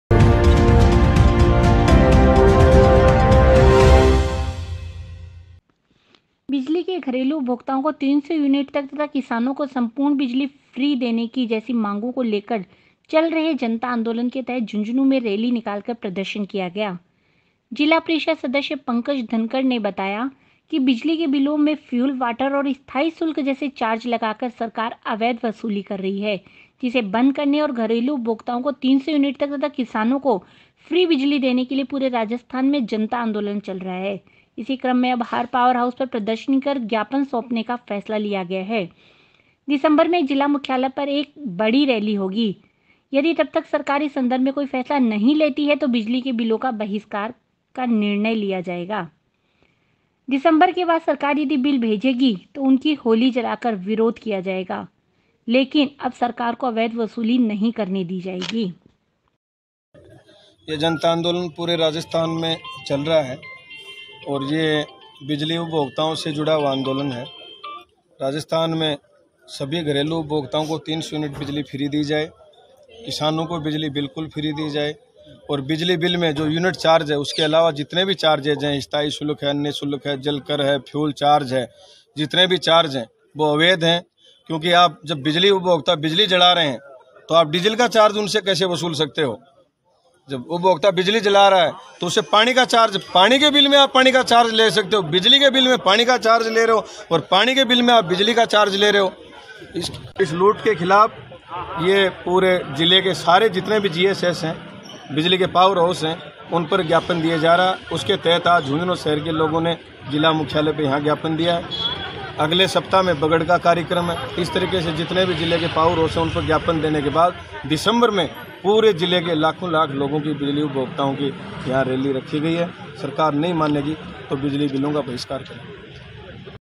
JHUNJHUNU I जनता आंदोलन के तहत रैली निकालकर सौंपा ज्ञापन
बिजली के घरेलु उपभोक्ताओं को 300 यूनिट तक तथा किसानों को संपूर्ण बिजली फ्री देने जैसी मांगों को लेकर चल रहे जनता आंदोलन के तहत झुंझुनूं में रैली निकालकर प्रदर्शन किया गया। जिला परिषद सदस्य पंकज धनखड़ ने बताया कि बिजली के बिलों में फ्यूल, वाटर और स्थायी शुल्क जैसे चार्ज लगाकर सरकार अवैध वसूली कर रही है। जिसे बंद करने और घरेलु उपभोक्ताओं को 300 यूनिट तक तथा किसानों को फ्री बिजली देने के लिए पूरे राजस्थान में जनता आंदोलन चल रहा है।